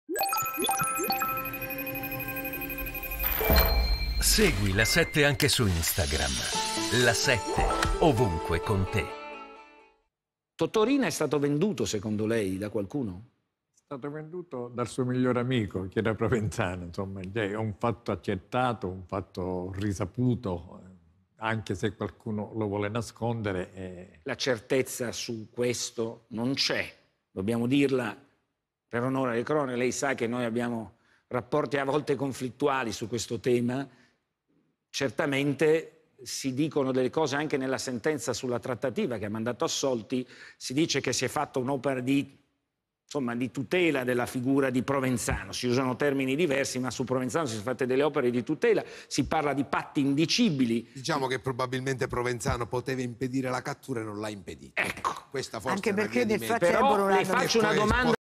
Il pentito Mutolo: "Riina venduto da Provenzano"
Il pentito Gaspare Mutolo: "Totò Riina è stato venduto dal suo miglior amico, Provenzano"